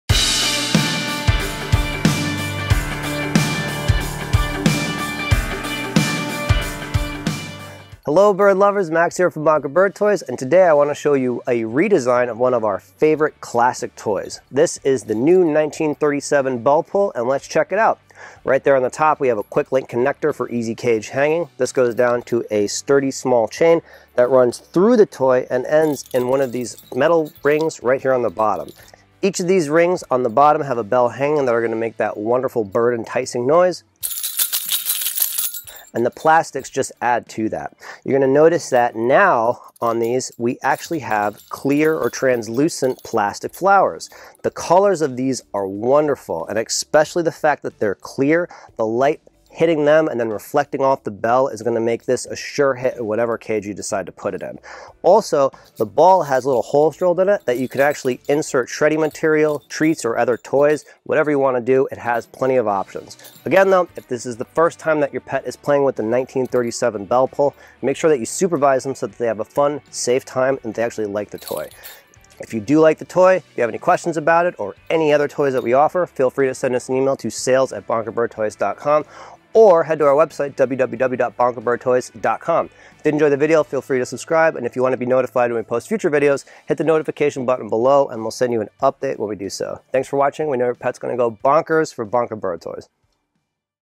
1937 Bell Pull Bonka Bird Toys
The 1937 Bellpull from Bonka Bird Toys is a ridiculously entertaining good time for your small to medium sized feathered friend! The bright colors and beak focused textures stand out in any cage, accenting the surroundings. The top of the 1937 has a quick link connector making hanging it simple. A metal chain hangs down from the connector, runs through the center of the toy and ends with a small metal ring on the bottom. This metal ring has a stainless steel bell attached. Strung through the ball there are two more chains that have stainless steel bells and plastic rings hanging from either side. When your pet pulls on one of these bells or rings the other side will slide up creating lots of exciting noises and movement. The center ball can be filled with shreddy and foraging material too! The 1937 Bellpull measure about (3) inches wide and (9) inches tall. It weighs about (2) ounces and is 100% bird safe.

The chain pulling effect will keep your bird entertained.
Brightly colored charms.
Plastic easy to clean.
Quick link for easy cage connection.